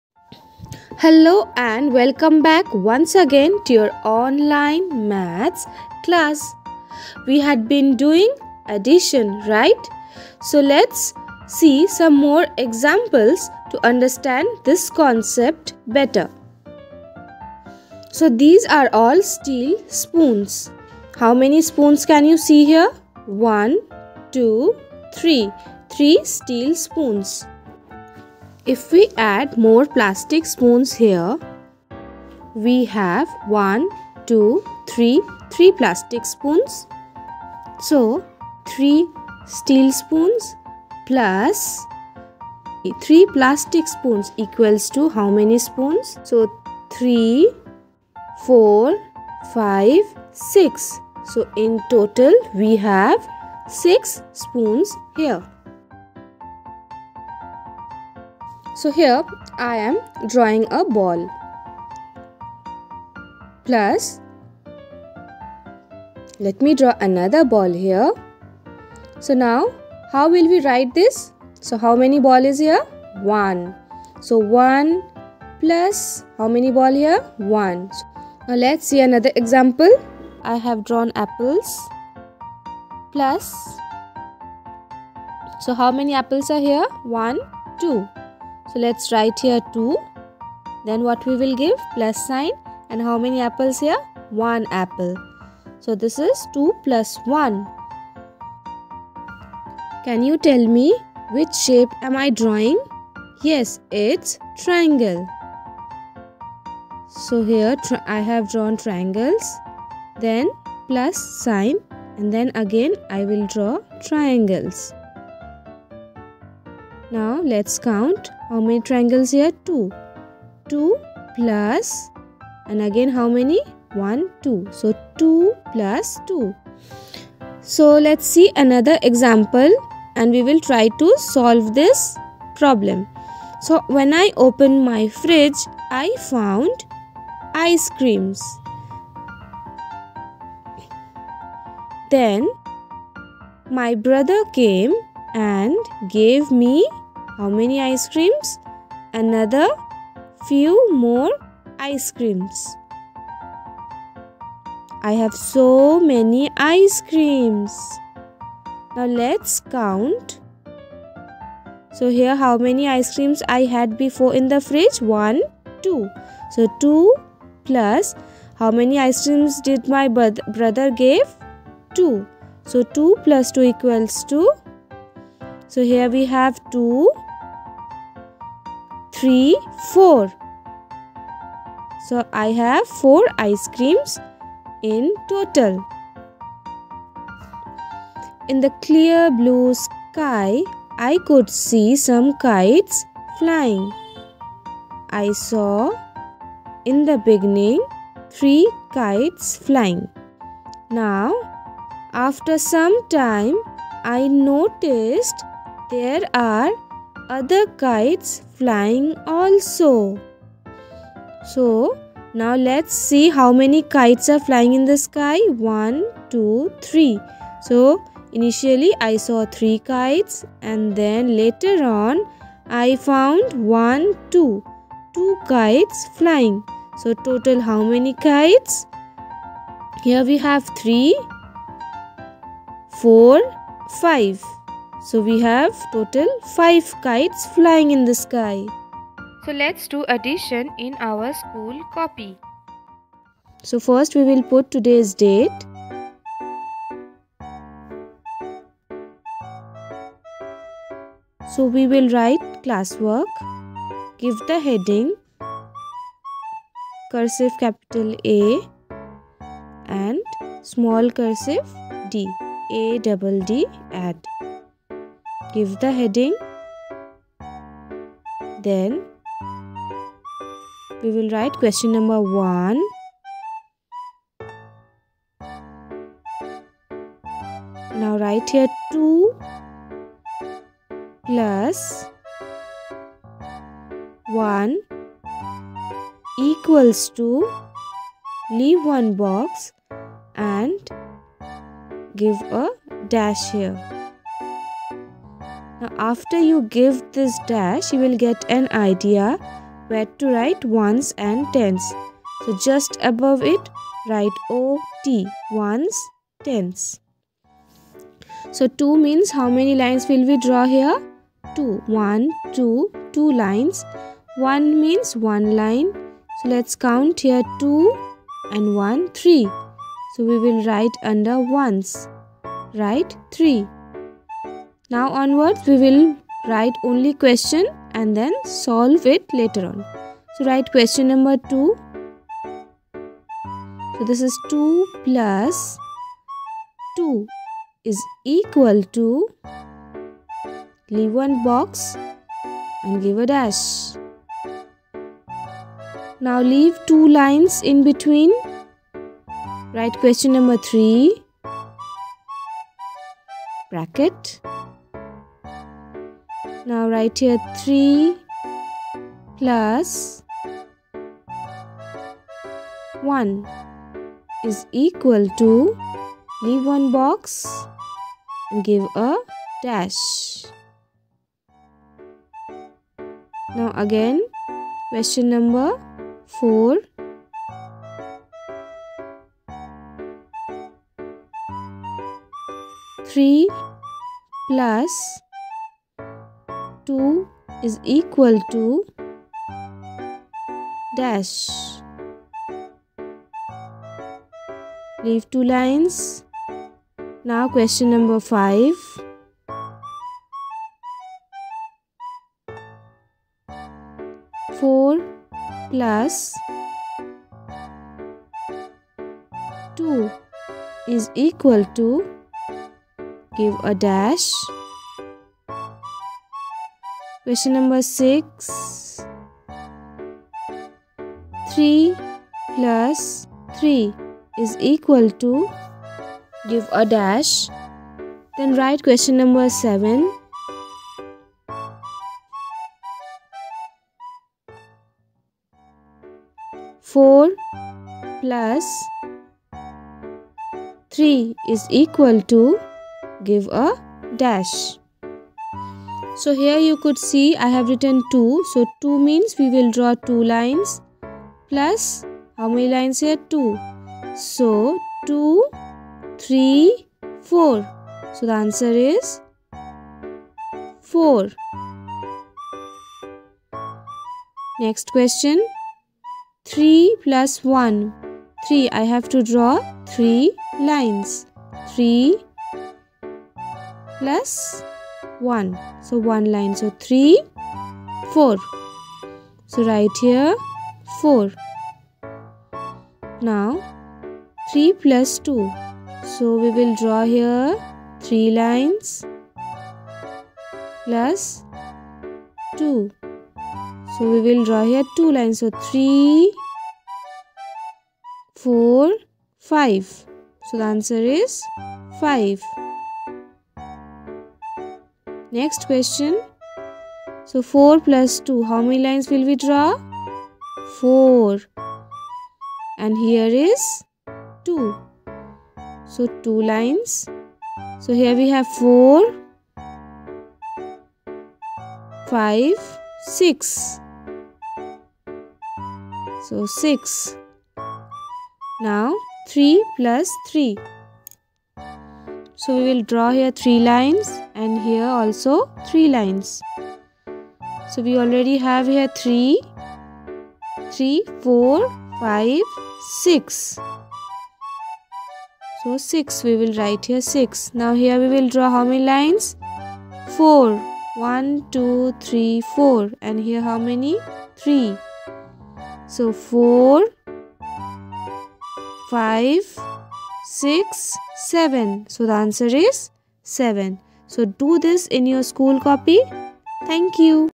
Addition (part - 3)
Kindergartners are developing a foundation that will equip them to learn for the rest of their academic lives and math skills are particularly important.

Addition helps kids master the relationships between numbers and understand how quantities relate to one another. Even when kindergartners can't reliably answer addition problems or manipulate large numbers, basic addition skills give them a framework for mastering math in elementary school.